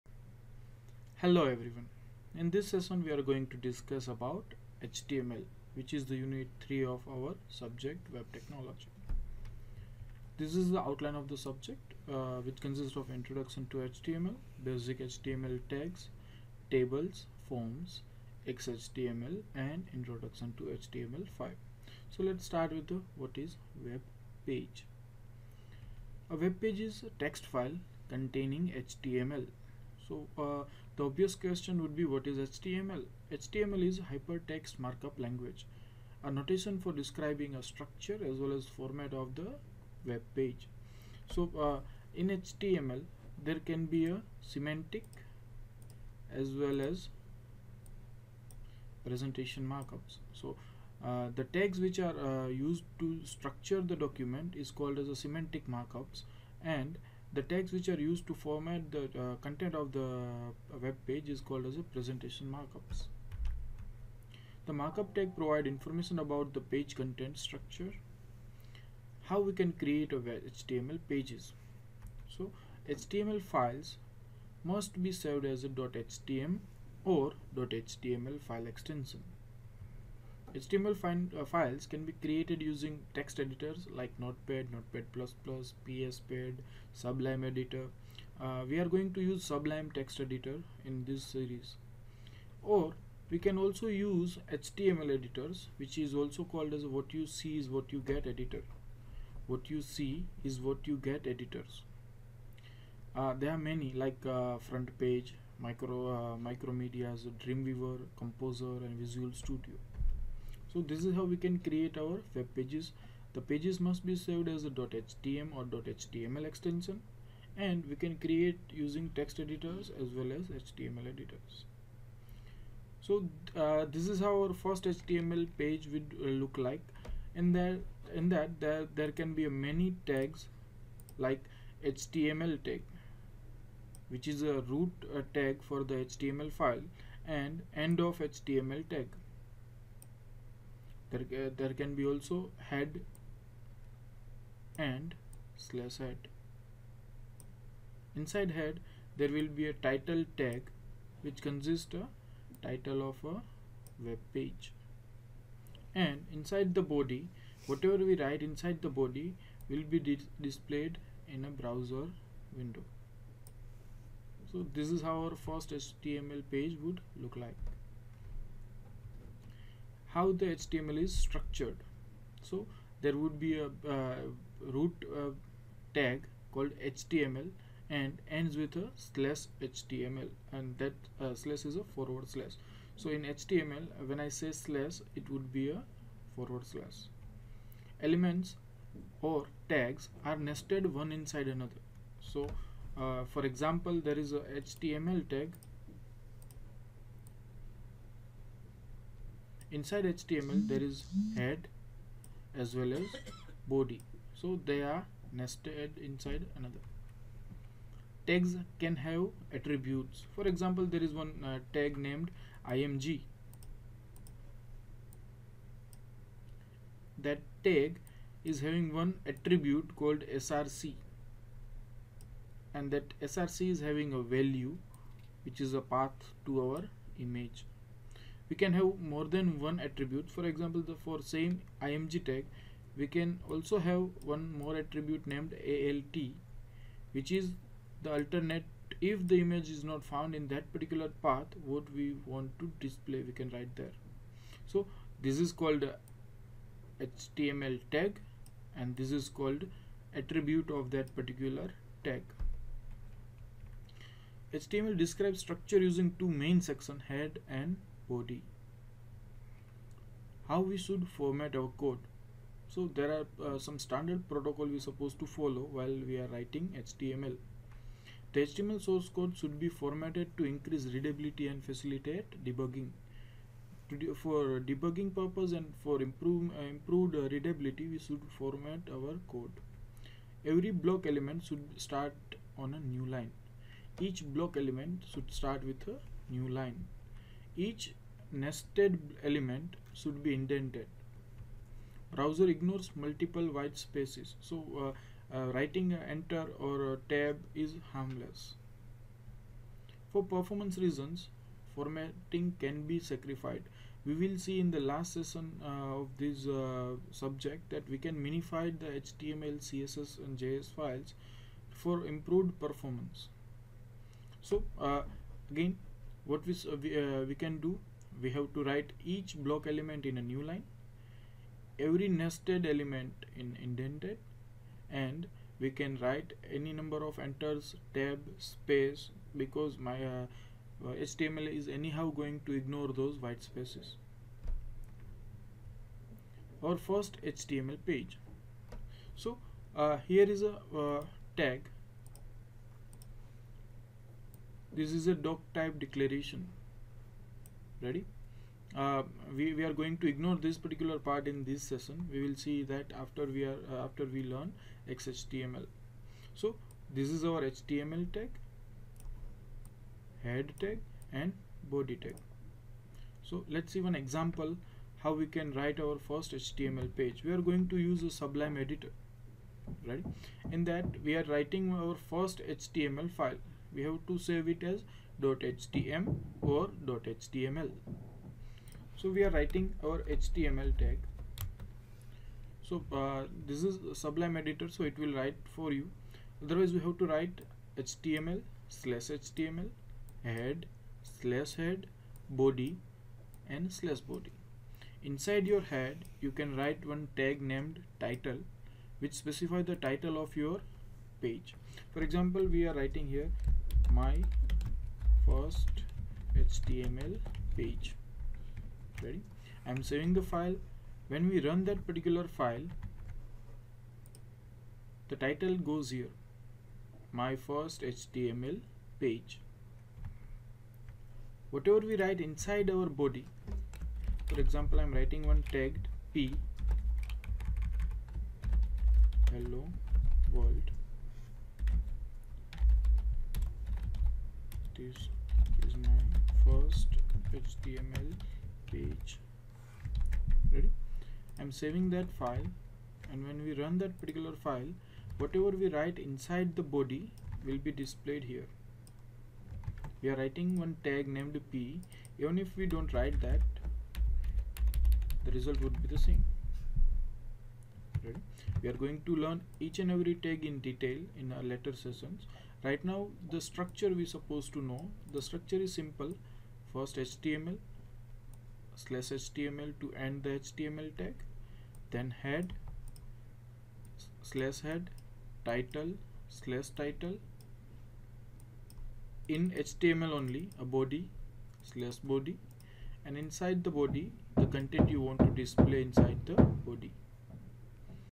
Unit 3 - Introduction to HTML
In this session, we are going to learn the basics of HTML. After this session, you can start creating basic HTML pages.

This video is recorded by Prof. Arjun V. Bala ( , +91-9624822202) at the Computer Engineering Department of Darshan Institute of Engineering & Technology, Rajkot.